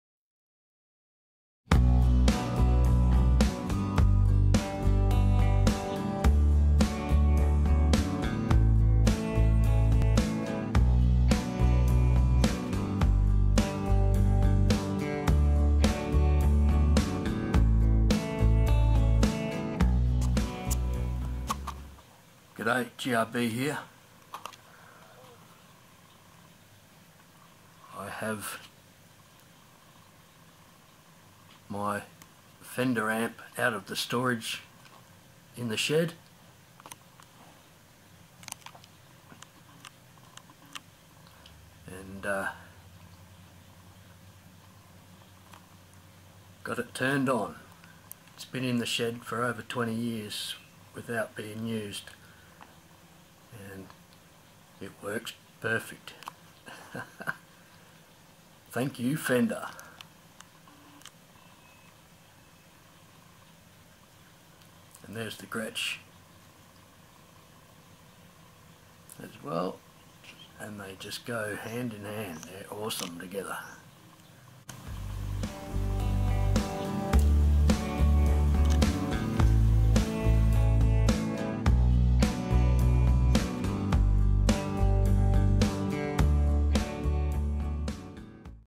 Introducing my Gretsch Guitar and Fender Amp.
Short Video Introducing my Awesome new (second hand Bargain) Gretsch Junior Jet Electric Guitar and my over 40 year old Fender Amp. The Amp has been in storage in my shed for over 20 Years and not used. Hope it works.
The Gretsch has just one "Gretsch Bucker" Pickup with Rosewood Fretboard that is just a Joy to play. I am Stoked. AWESOME.